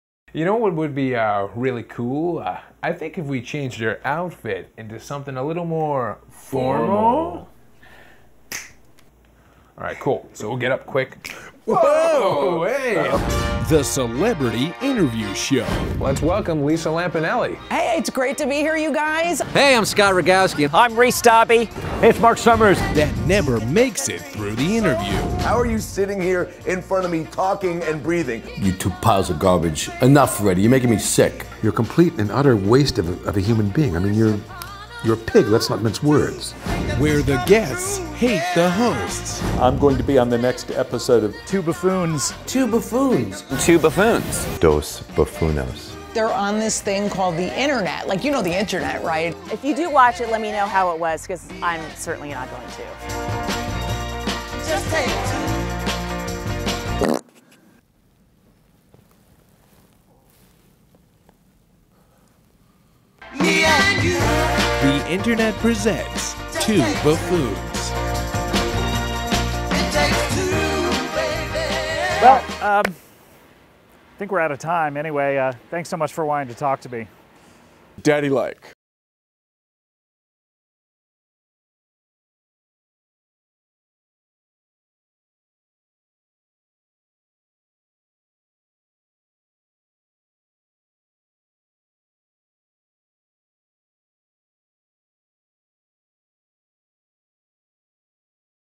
2 BUFFOONS TRAILER
2 BUFFOONS CHANNEL TRAILER
THE CELEBRITY INTERVIEW SHOW THAT NEVER MAKES IT THROUGH THE INTERVIEW